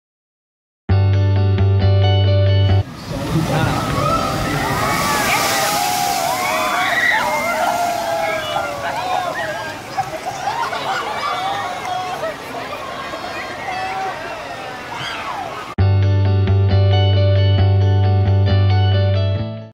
Tidal Wave at Thorpe Park
This is the Tidal Wave Ride at Thorpe Park.

We all got wet from this side but it was all good fun!